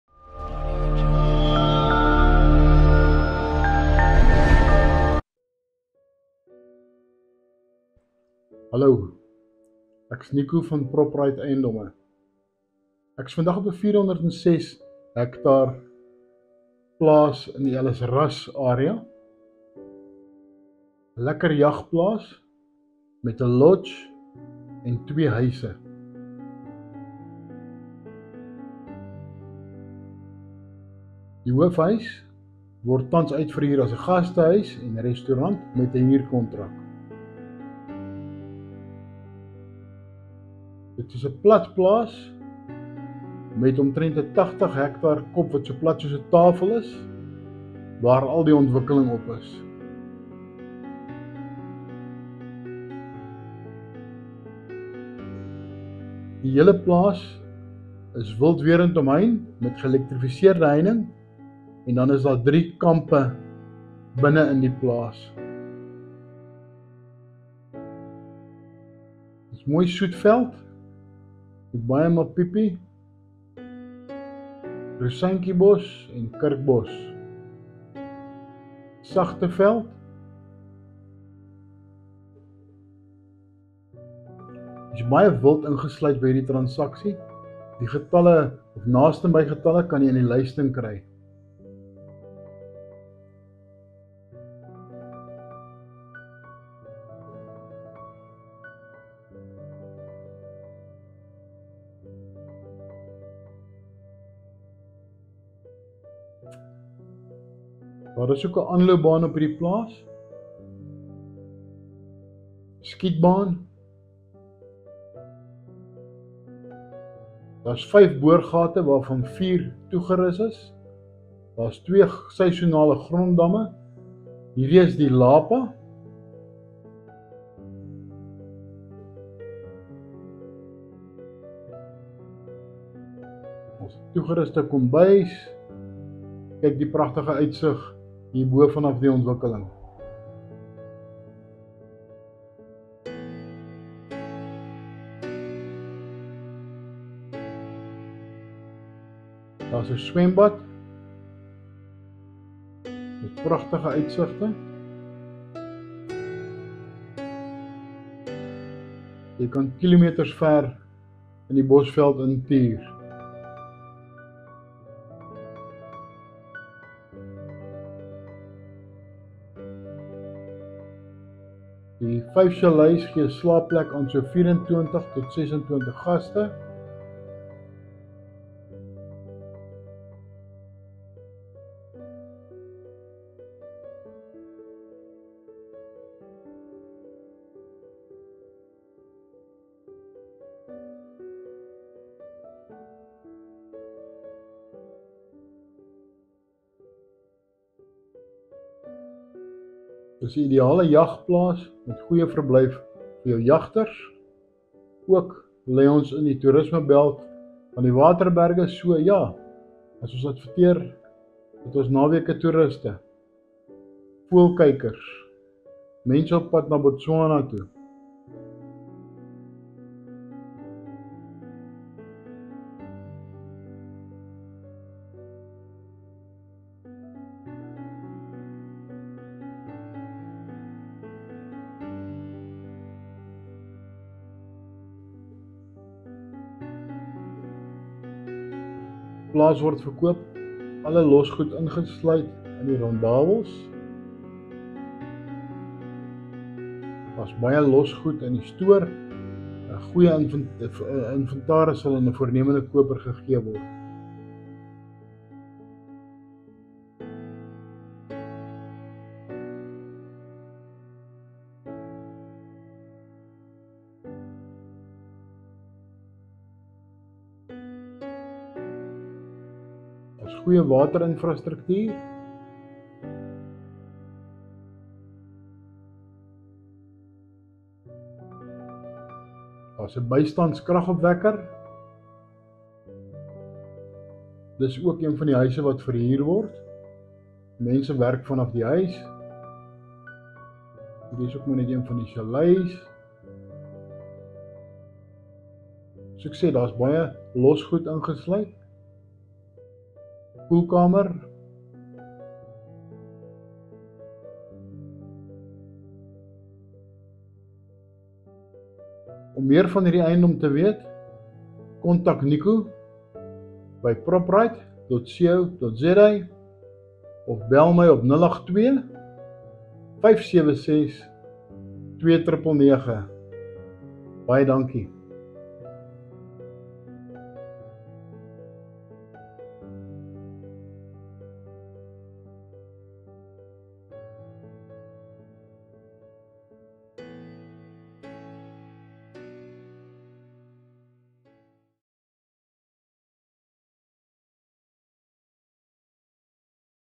PR1406 Game Farm for sale Ellisras Lephalale 406 ha .
This Farm is situated about 20 km from Ellisras

320 Km From Pretoria

Two houses are being rented out on a contract income of R17500 per month

Lodge sleeps 24 to 26 Guests

The lodge has Lapa, kitchen, dining area pool, and jacuzzi.

Runway to land a small plane.

Coldroom and Butchery

Ideal hunting farm

Inside farm

Flat farm with 80 ha flat table type koppie

Breathtaking views

No Claims

5 Calets

3 Sheds

Workers accommodation

Fencing:

3 camps

electrified fencing around the farm

2 seasonal earth dams

5 boreholes 4 equipped

Game Numbers may vary:

Impala 400

Bluwildebeest 60

Waterbuck 15

Kudus 25

Eland 25

Zebra 15

Nyalas 15

Giraffe 3

Inventory of the loose assets available for serious buyers

Seller registered for Vat

Only financially capable buyers!!!